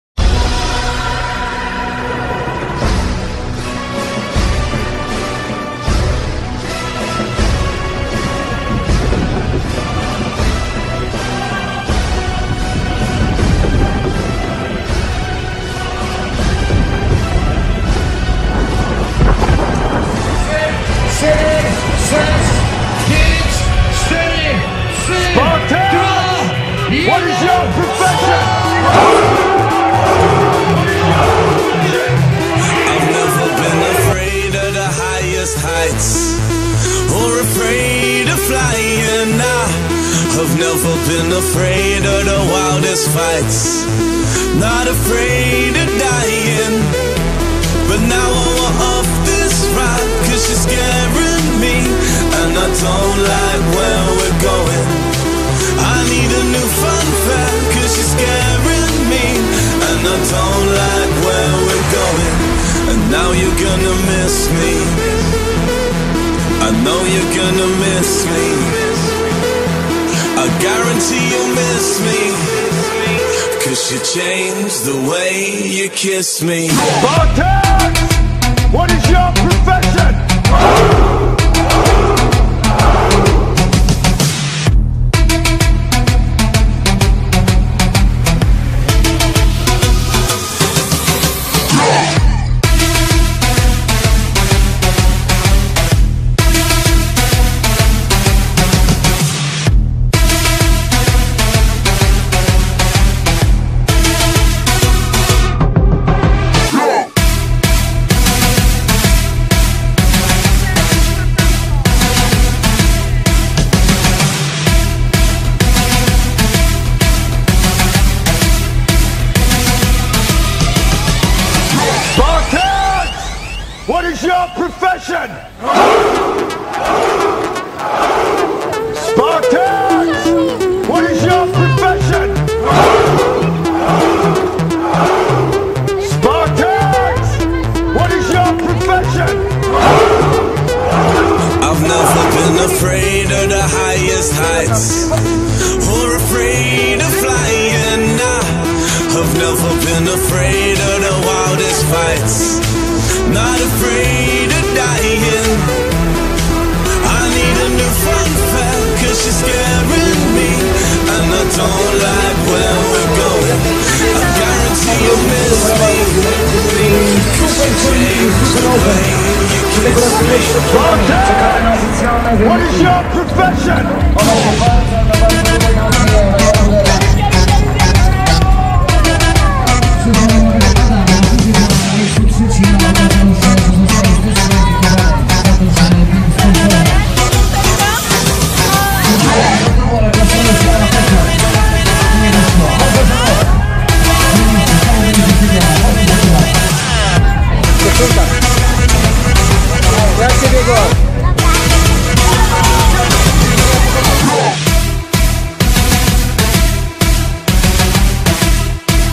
Power Oliwia Spartan Race Kids Krynica Zdrój Jaworzyna 30.07.2017
Tym razem krótka relacja, ale jest ;)